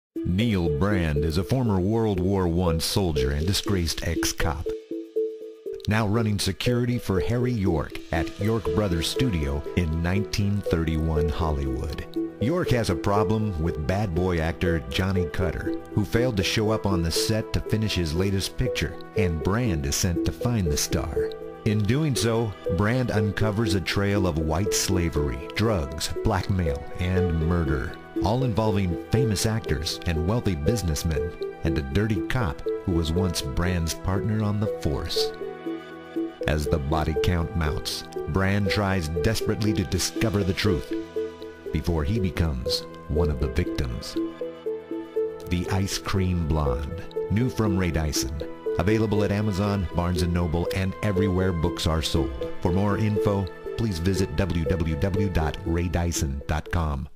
Book Trailer: The Ice Cream Blonde
Neil Brand is a former World War I soldier and disgraced ex-cop, now running security for Harry York at York Brothers Studio in 1931 Hollywood. York has a problem with bad boy actor Johnny Cutter, who failed to show up on the set to finish his latest picture, and Brand is sent to find the star. In doing so, Brand uncovers a trail of white slavery, drugs, and murder, involving famous actors and wealthy businessmen—and a dirty cop who was once Brand’s partner on the force. As the body count mounts, Brand tries desperately to discover the truth—before he becomes one of the victims.